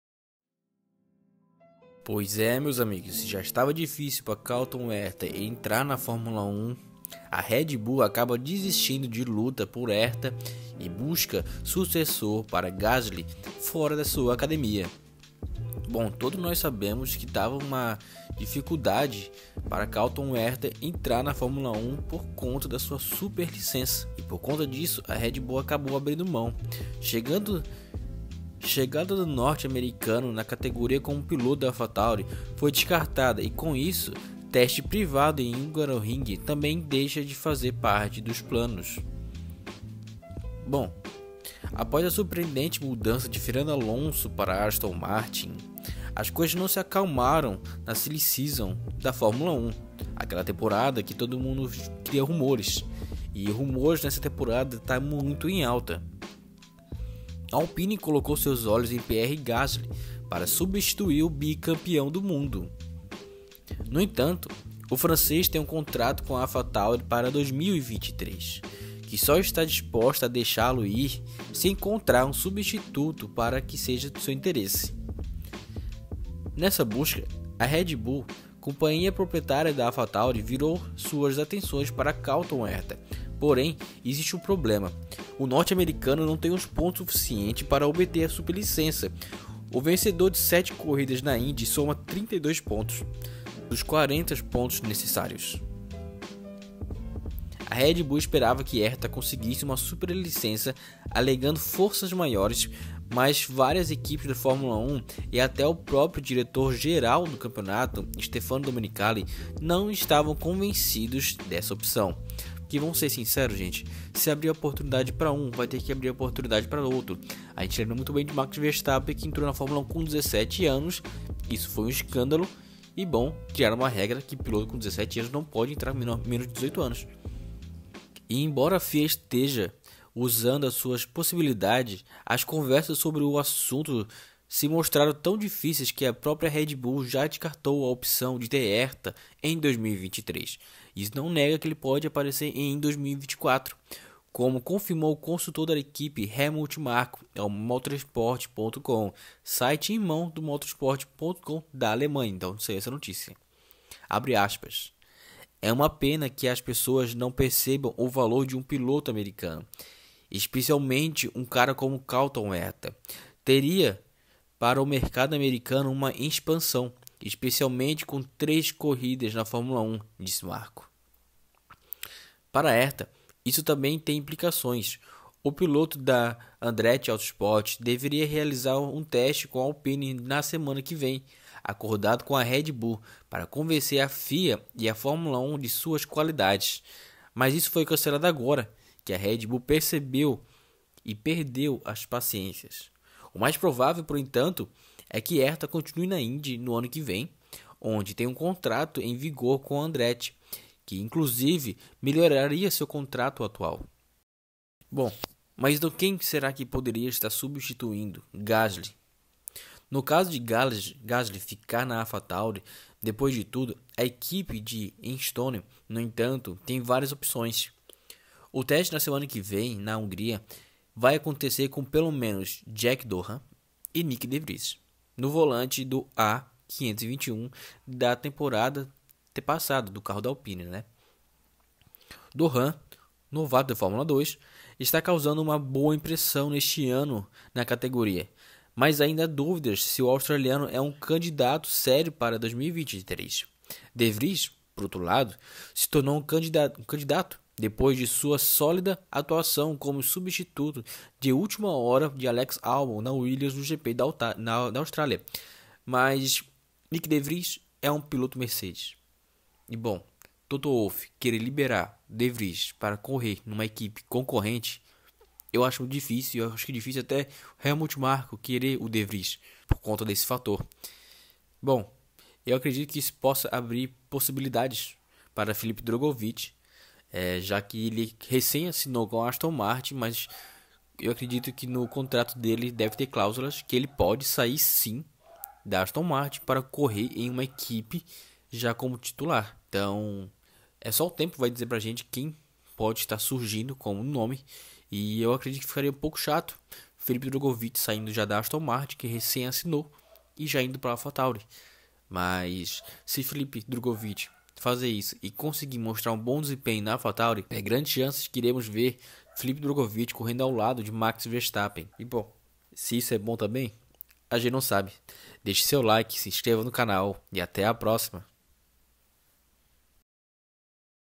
RED BULL DESISTE DE COLTON HERTA | REAL CHANCES PARA DRUGOVICH?
Chegada do norte-americano na categoria como piloto da AlphaTauri foi descartada e com isso, teste privado em Hungaroring também deixa de fazer parte dos planos

A Alpine colocou seus olhos em Pierre Gasly para substituir o bicampeão mundial. No entanto, o francês tem contrato com a AlphaTauri para 2023, que só está disposta a deixa-lo ir se encontrar um substituto que a interesse.

Nessa busca, a Red Bull, companhia proprietária da AlphaTauri, virou suas atenções para Colton Herta. Porém, existe um problema: o norte-americano não tem pontos suficientes para obter uma superlicença, o vencedor de sete corridas na Indy soma 32 dos 40 necessários.

A Red Bull esperava que Herta conseguisse uma superlicença alegando "força maior", mas várias equipes de Fórmula 1 e até o próprio diretor geral do campeonato, Stefano Domenicali, não estavam convencidos dessa opção.